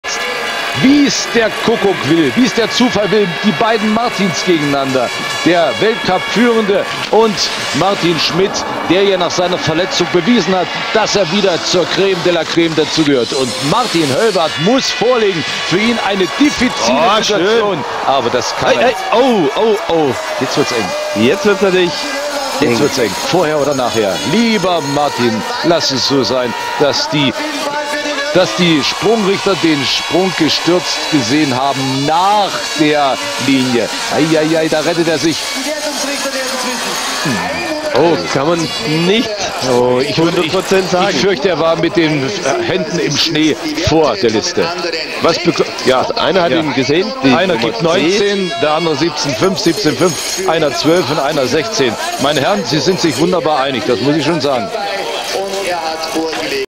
Martin Hoellwarth - 121m - CRASH - Ga-Pa 01.01.2003 K115 - Ski Jumping - World Cup - 4 Hills
Martin Hoellwarth got the following style marks: 17.5, 17.5, 12, 16 and 19.

Ski Jumping - World Cup - 4 Hills Tournament - Season 2002/2003

Martin Hoellwarth - 121m - 1st round - CRASH - Garmisch-Partenkirchen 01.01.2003 K115 - Ski Jumping - World Cup - 4 Hills Tournament